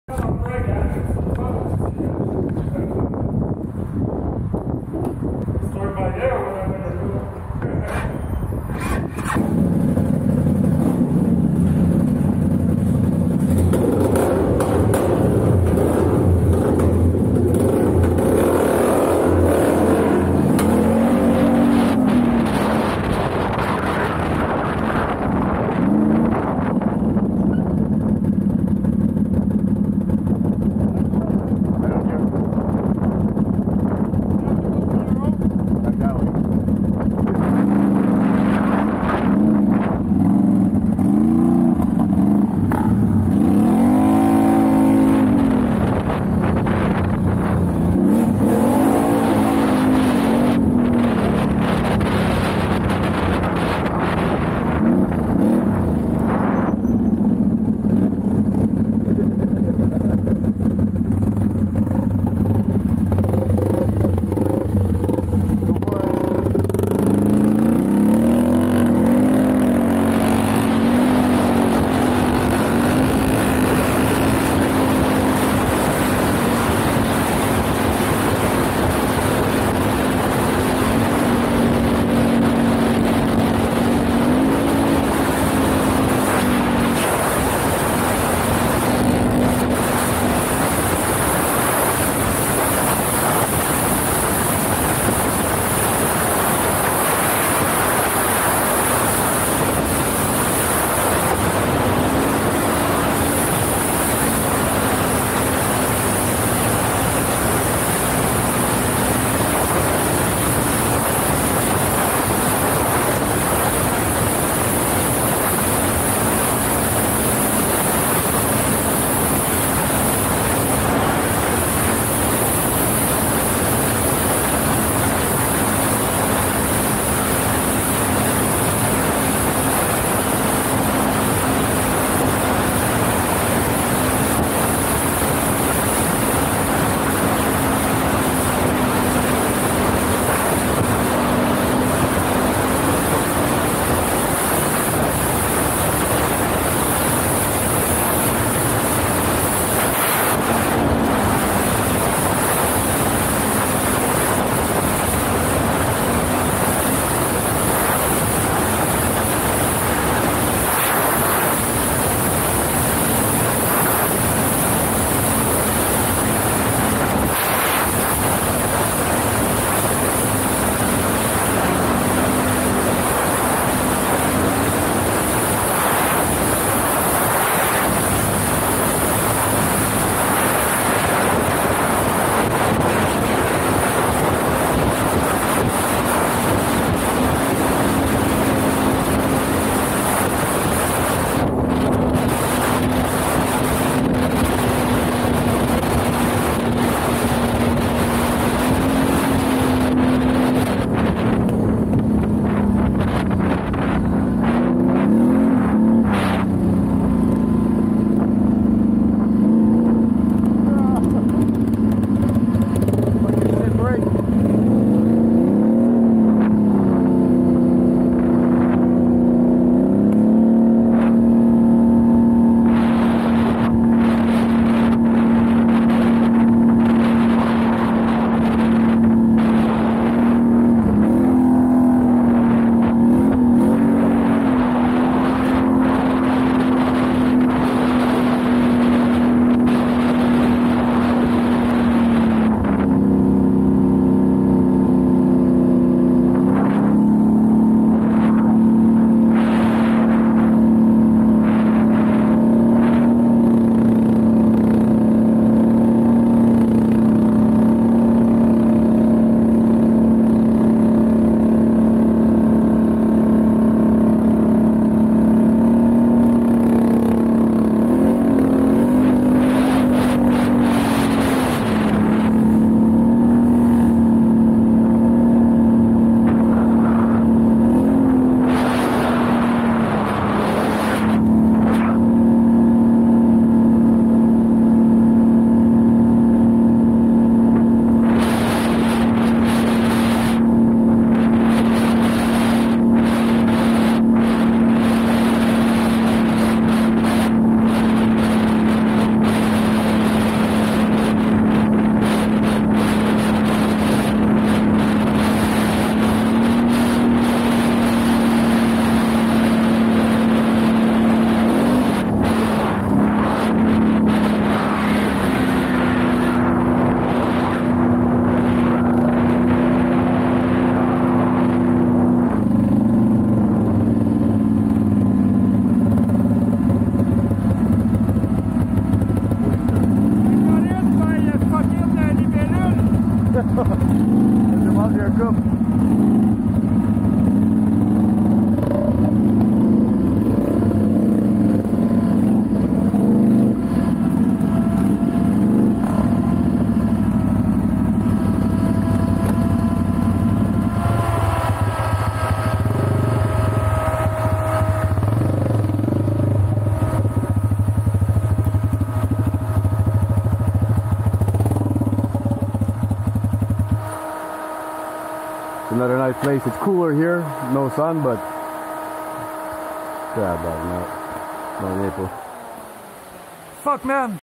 Little Speed Run with Phillipe Summer 2021 Some XMR Fun PART 2
He just picked up this old Honda 250 for cheap and decided to take it for a quik ride.
Still no match for my XMR 1000. Literaly Left him in the dust more than once. loll
Cheers